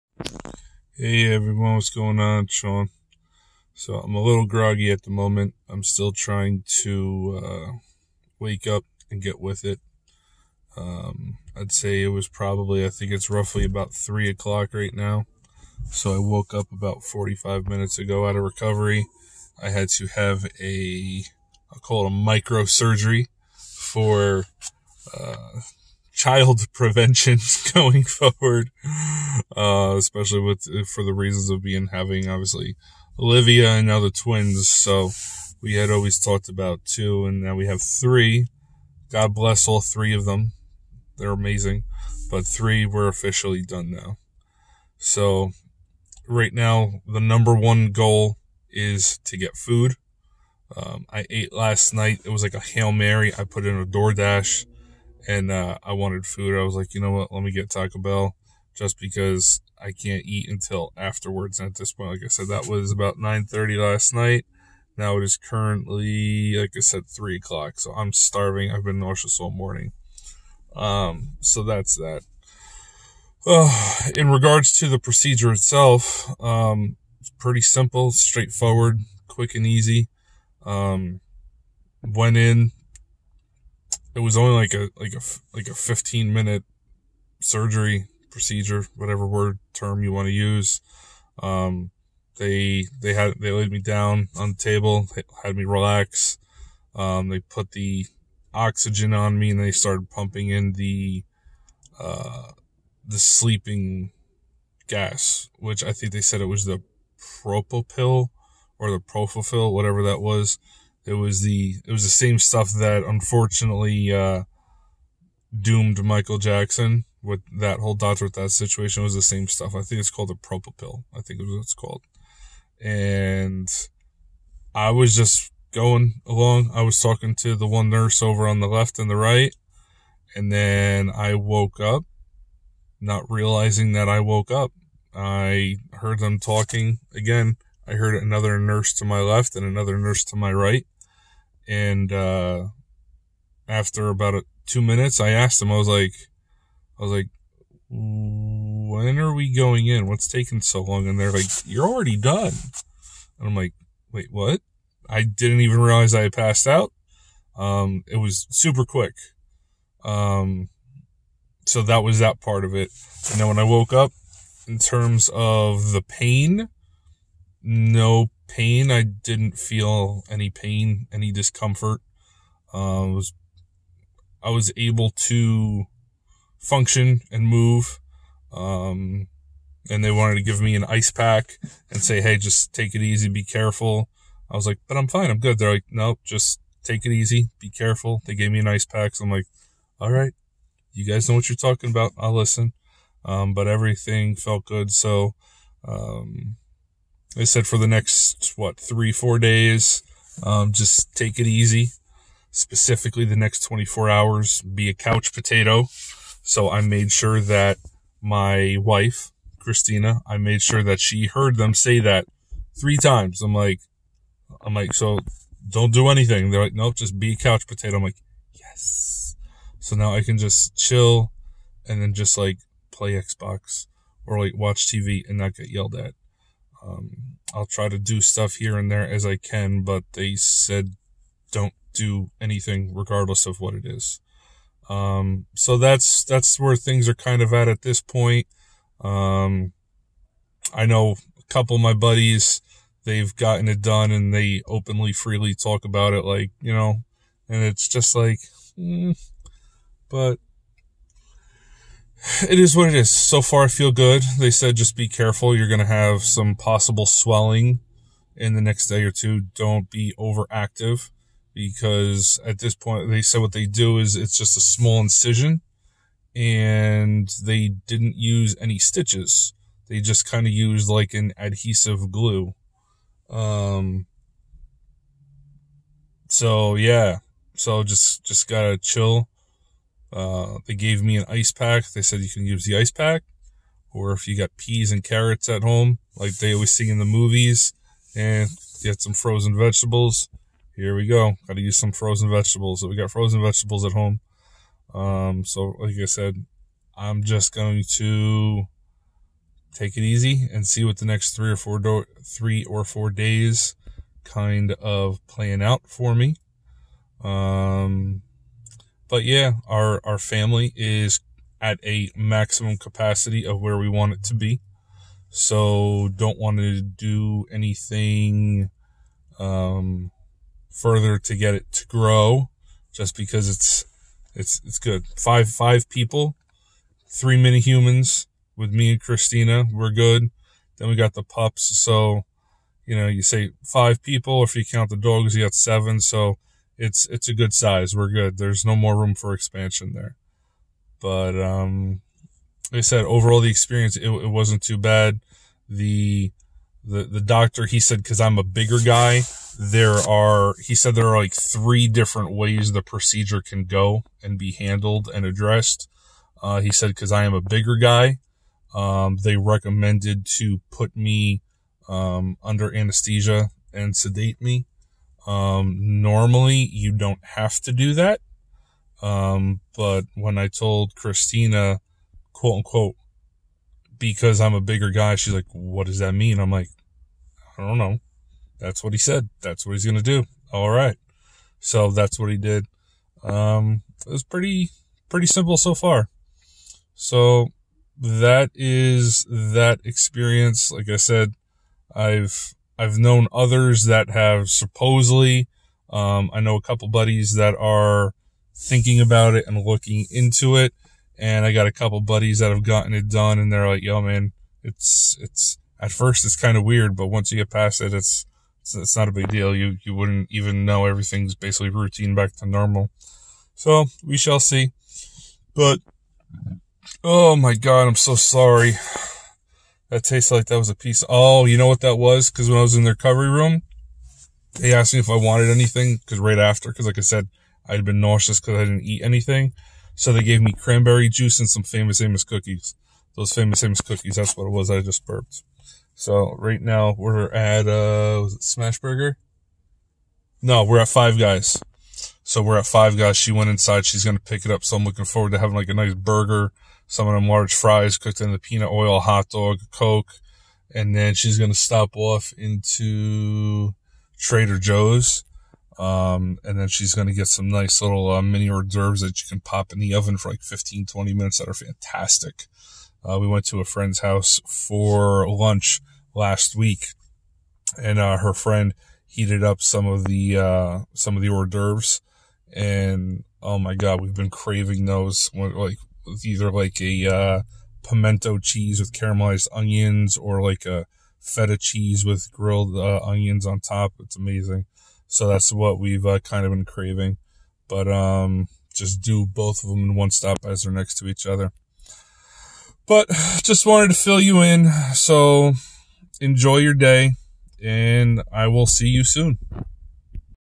Vasectamy post recovery
These are my immediate thoughts after the procedure.  All are good so far